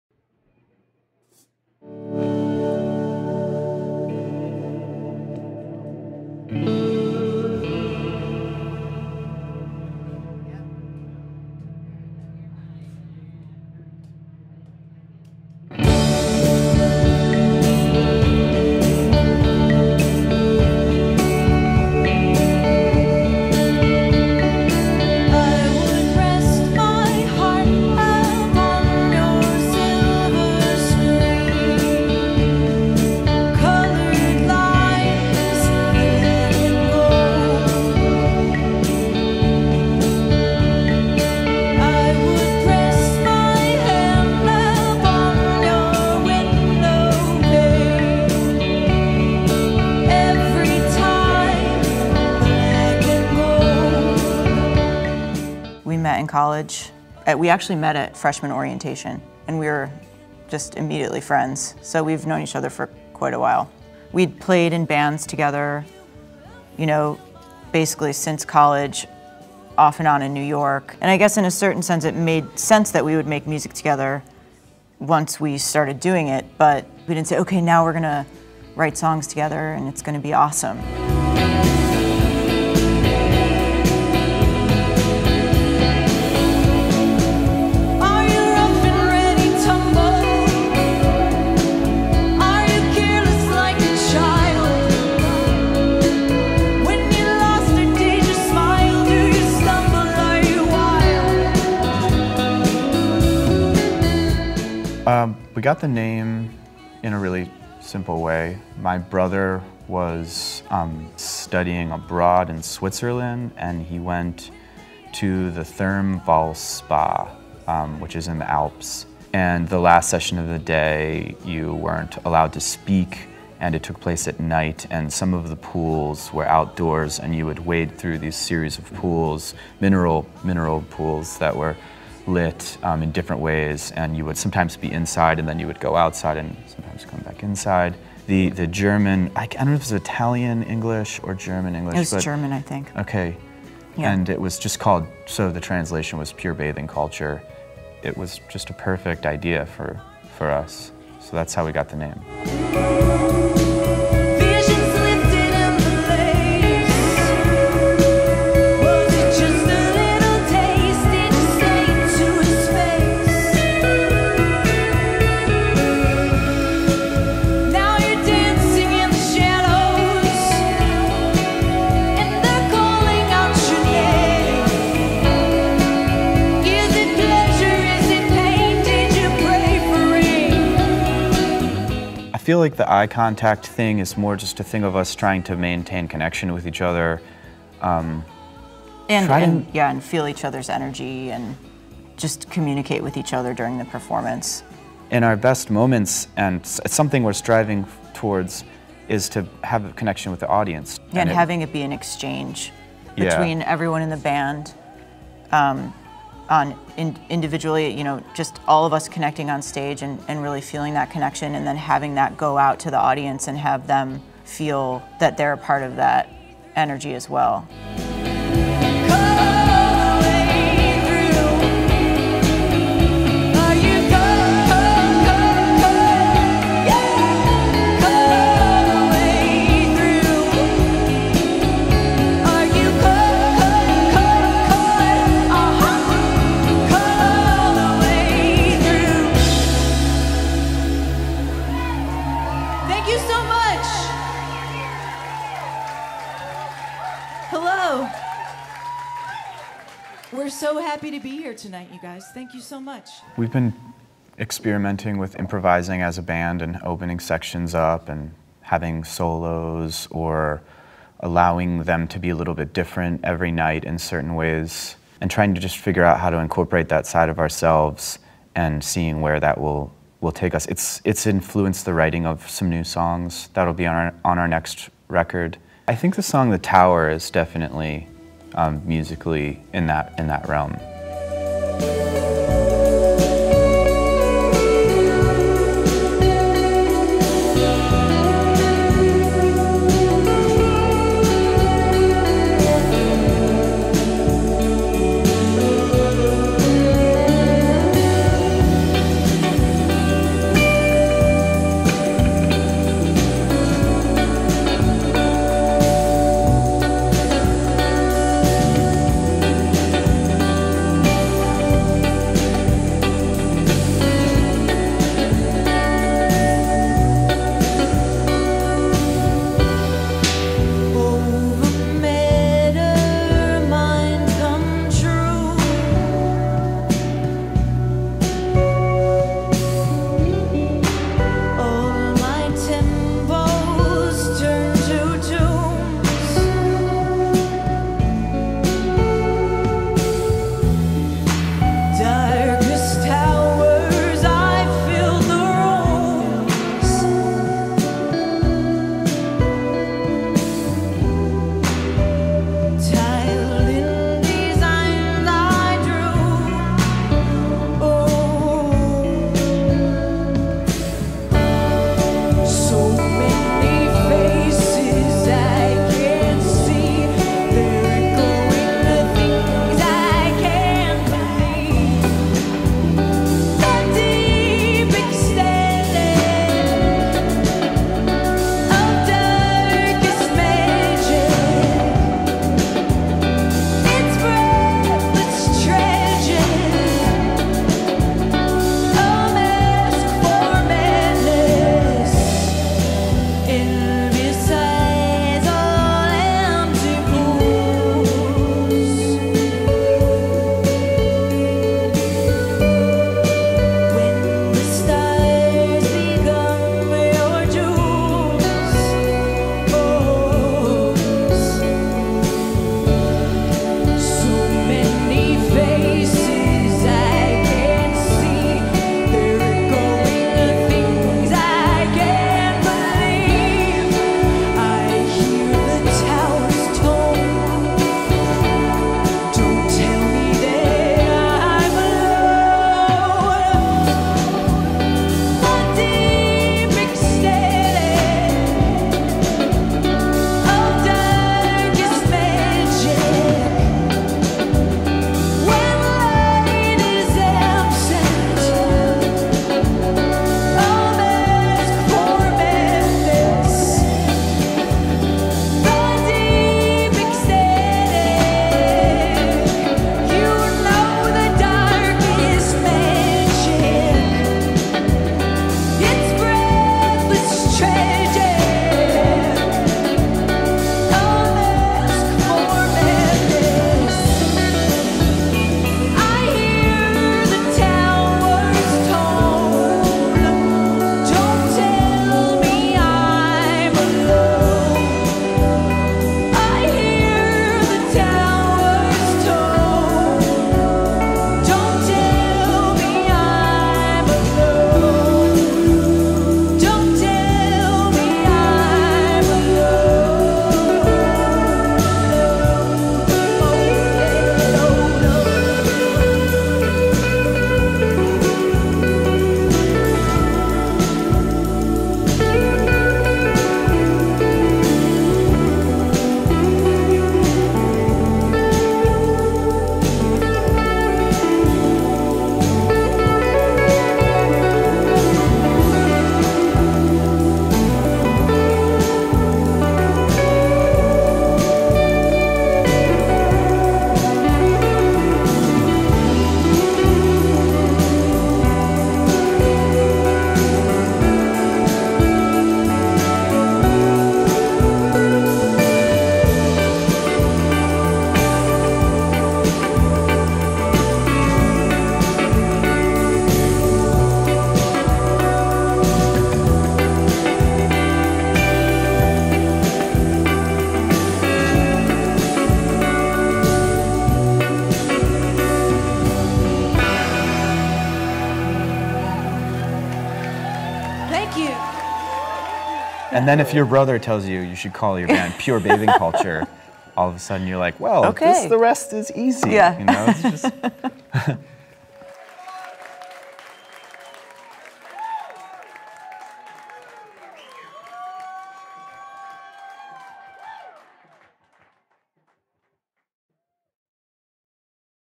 Pure Bathing Culture - Performance and Interview (opbmusic)
For opbmusic’s 10th anniversary this year, we’ve dug into the archives for some great performances from Stagepass, including this set from Pure Bathing Culture.